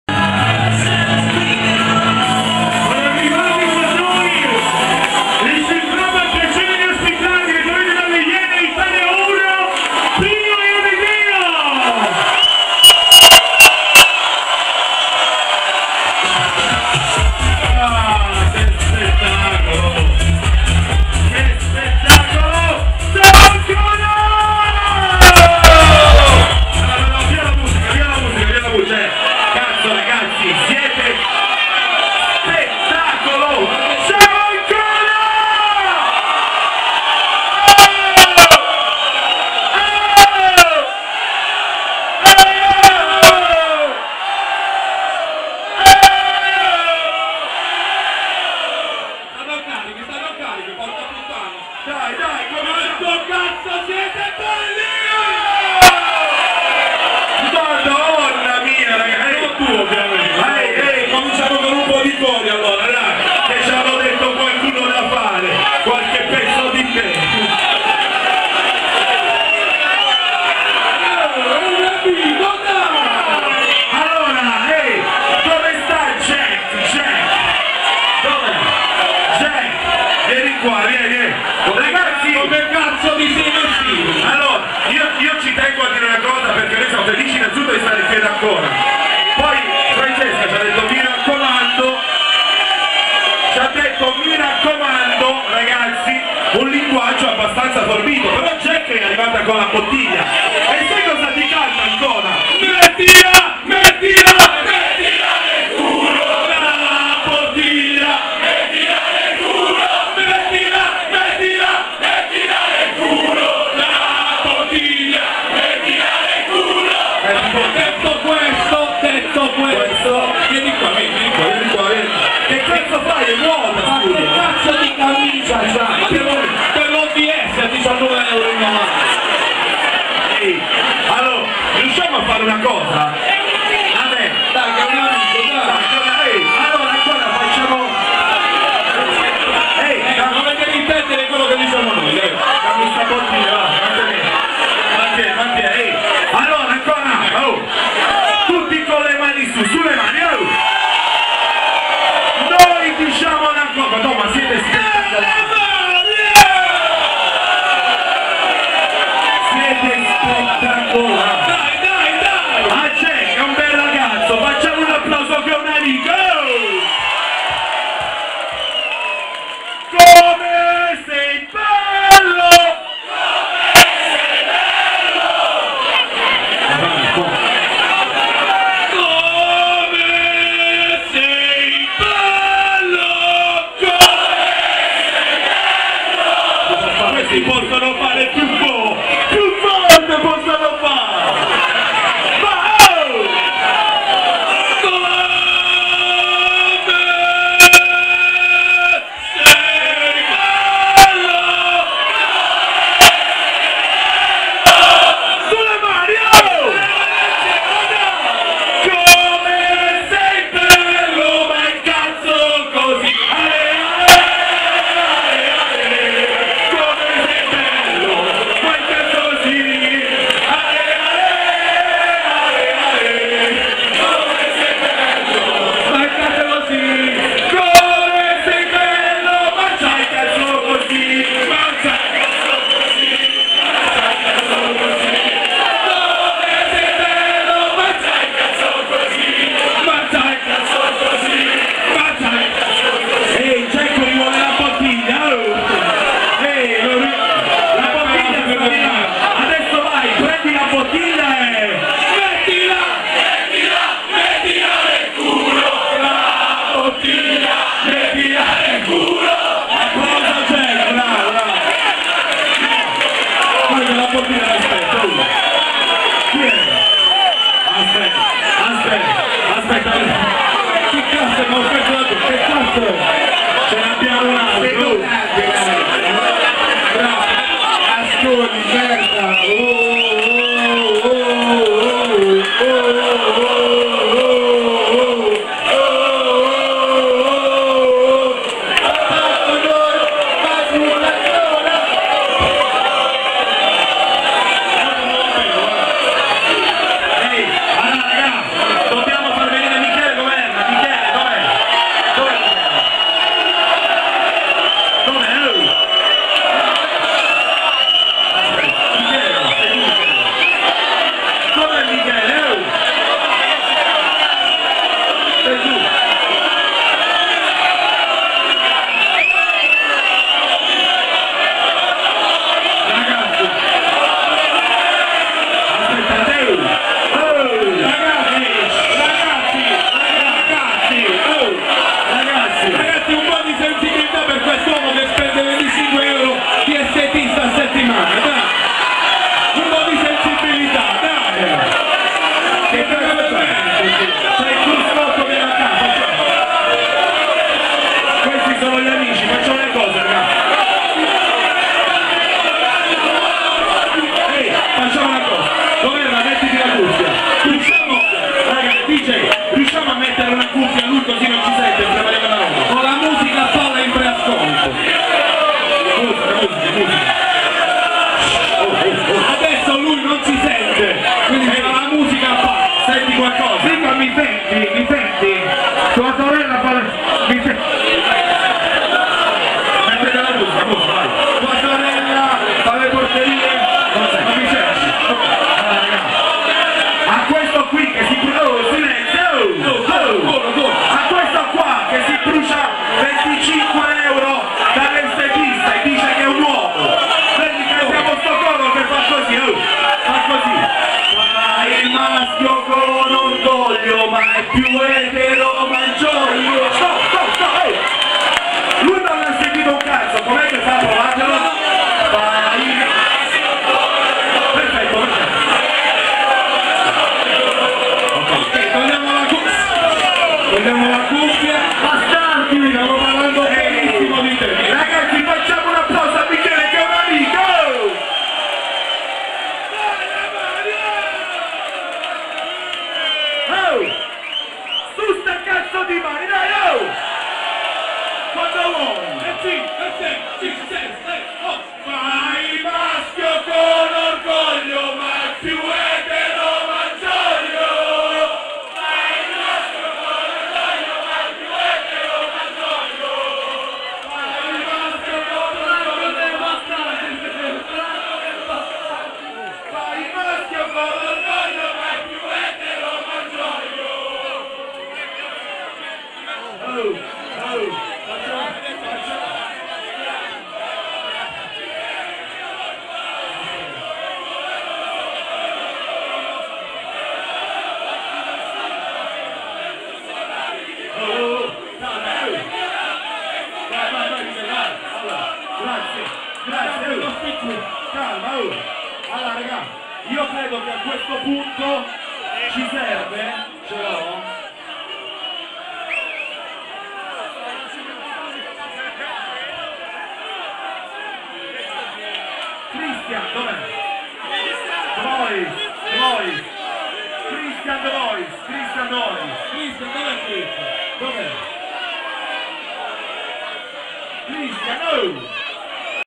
Pio & Amedeo al sui club di Ancona video 3
Pio & Amedeo fanno il loro ingresso al sui club di Ancona..
..un delirio..
un sentito ringraziamento a Francesca..
coll.artistica Bo.St management